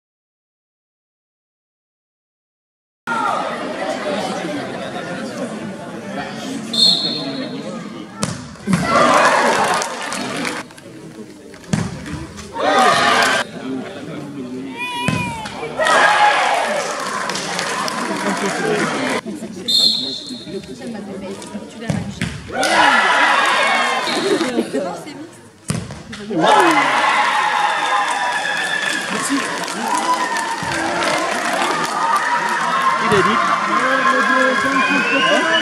Pénalty Villettes - Montregard
La séance de pénaltys entre Montregard et Les Villettes qualificative pour la finale !